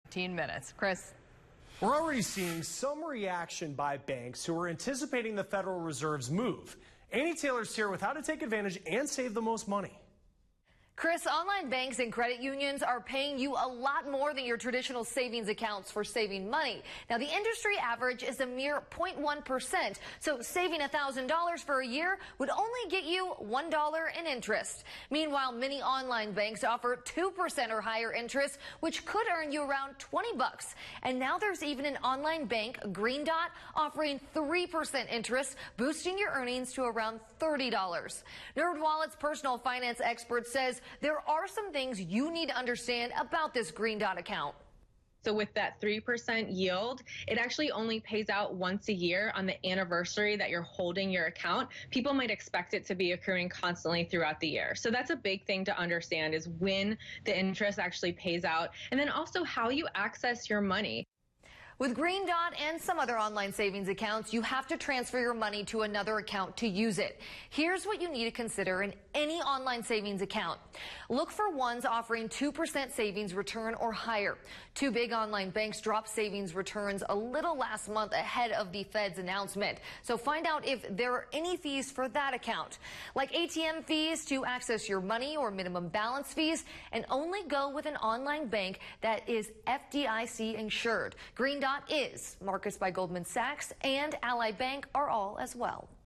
Online savings account offers 3% interest
NerdWallet's personal finance expert, Kimberly Palmer says online savings accounts are a great tool to earn extra on your savings but read the fine print.